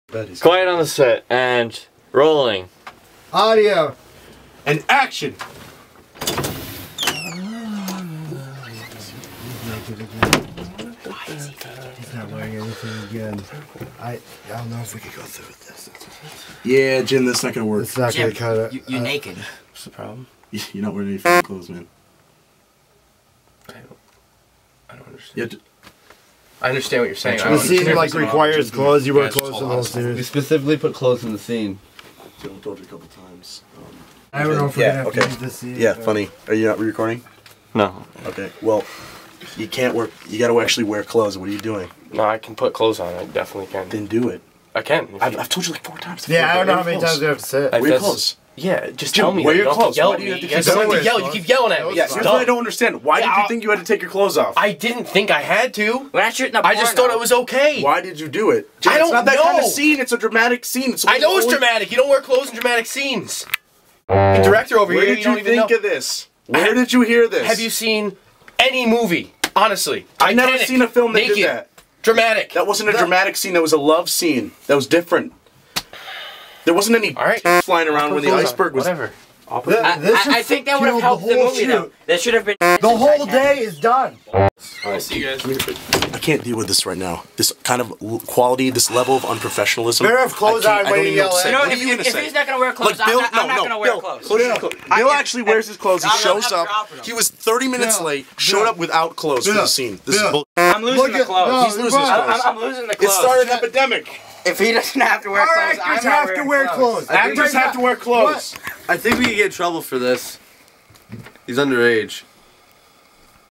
Behind the scenes of Hansford
When Jim Scannapieco shows up on set, disaster strikes.